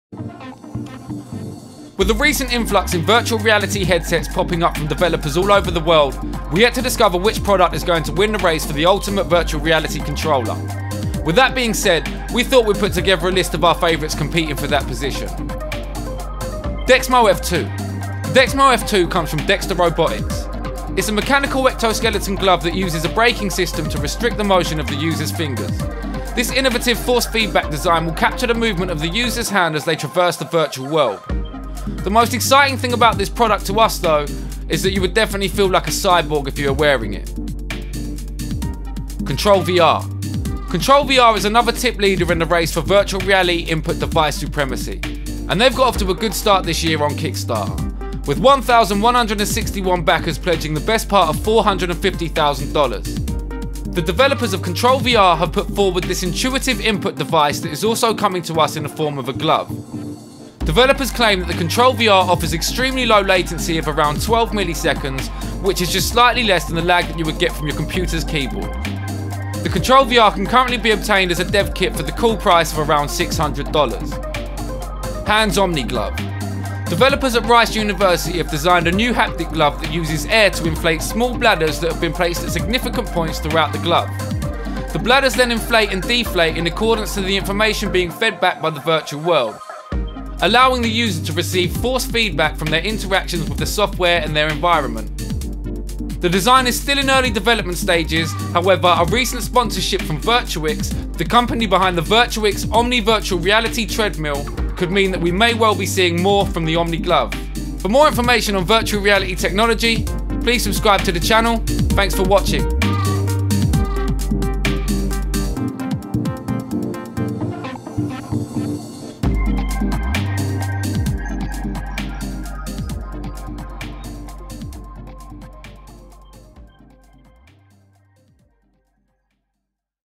Top 3 Virtual Reality Gloves Of The Future (Tech Gadgets)
Here's our top 3 Virtual Reality gloves, as of 1st may 2015.

3. Dexmo F2
2. Control VR
1. Hands Omni Glove